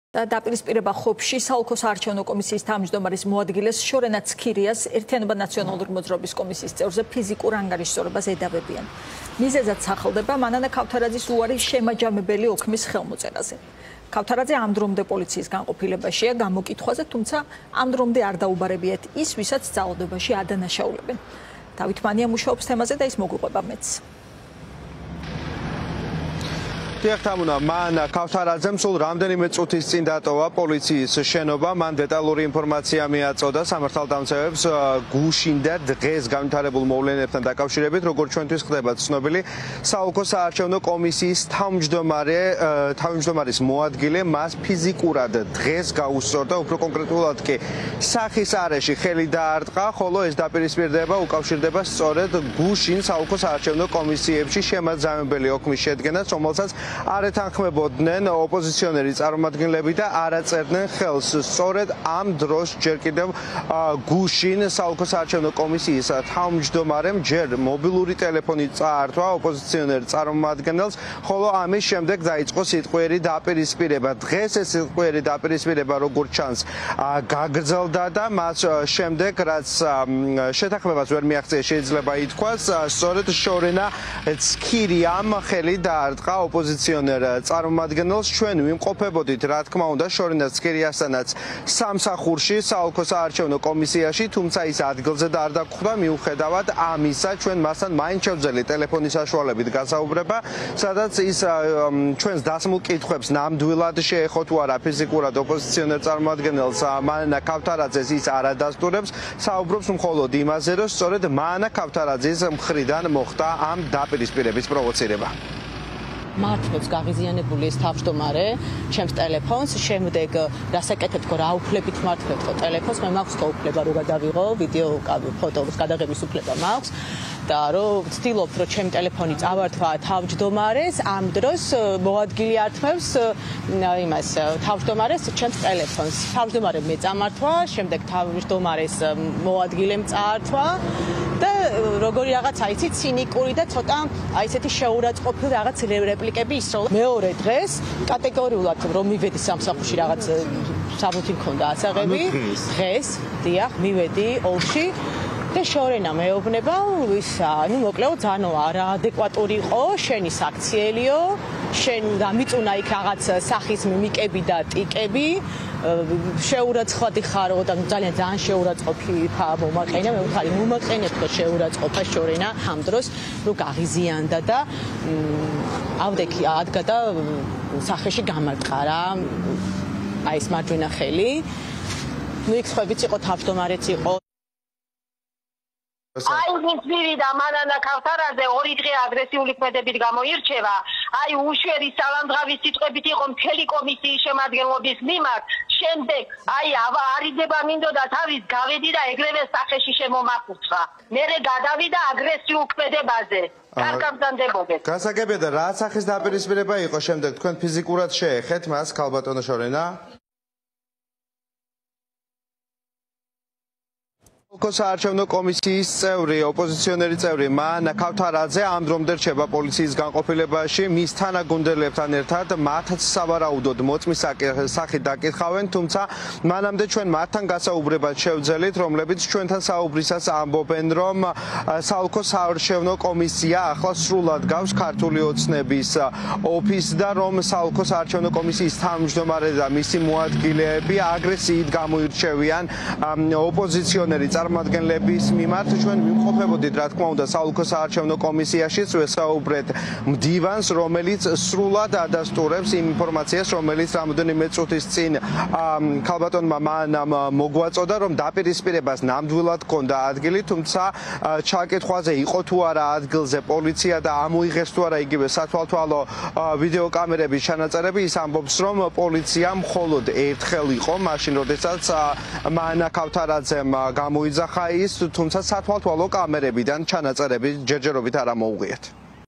დაპირისპირება ხობში
▪️ დაპირისპირება ხობში. საოლქო საარჩევნო კომისიის თავმჯდომარის მოადგილეს შორენა ქირიას ენმ-ის კომისიის წევრზე ფიზიკურ ანგარიშსწორებას ედავებიან. მიზეზად სახელდება მანანა ქავთარაძის უარი შემაჯამებელი ოქმის ხელის მოწერაზე